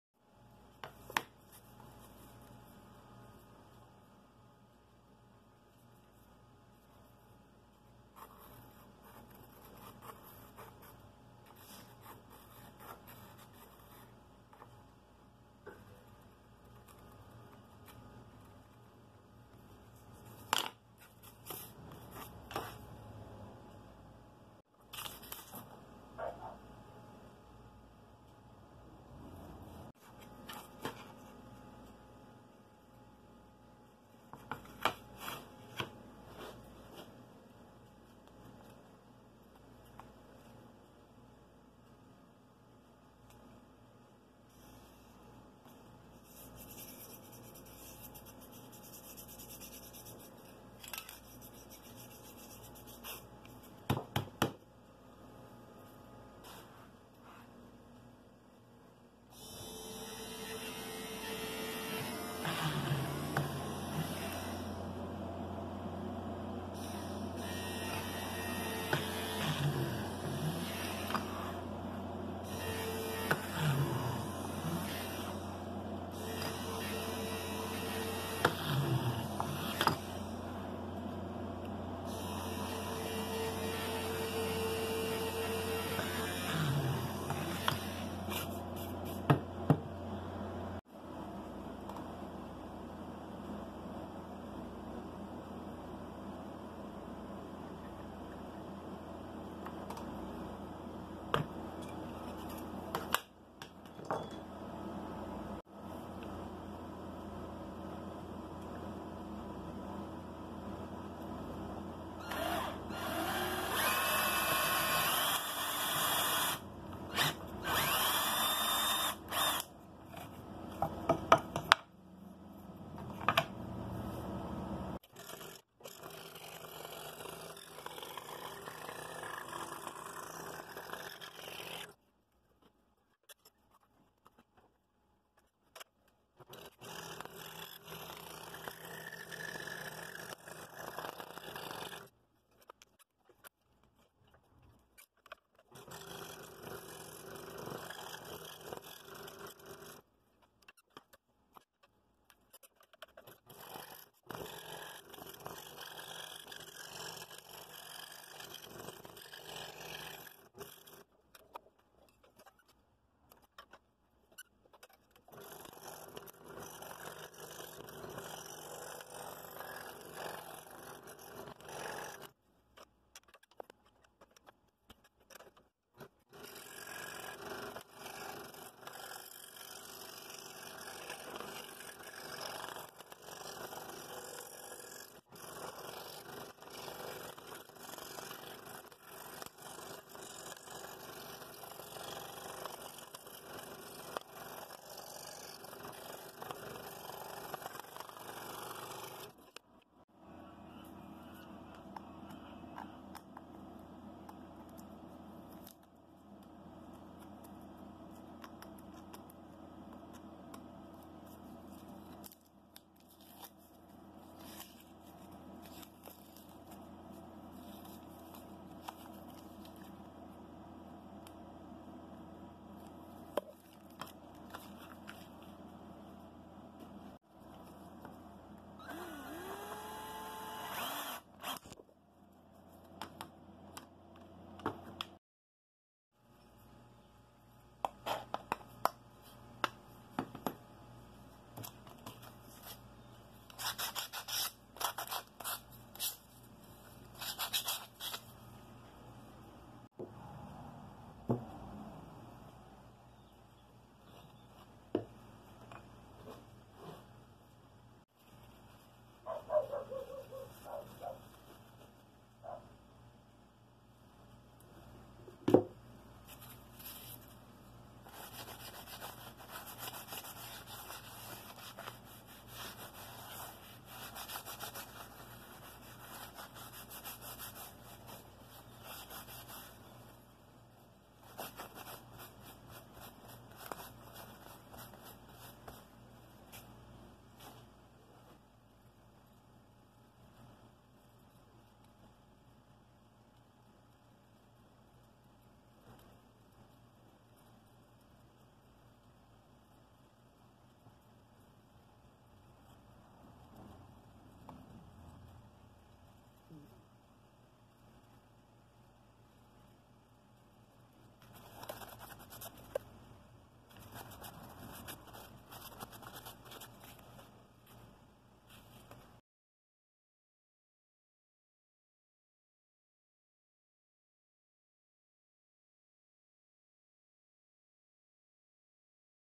A Simple Scroll Saw Pattern Alteration
A friend asked in the comments if a receptacle cover could be done in the same way.  So I suggested a possible alteration to do it and then did it.  This is the video of that.  I did it very simply by tracing a receptacle cover over the pattern.  You could import the pdf of the pattern into a vector graphics program, like Inkscape, and alter it that way.  But, this is much simpler and old school.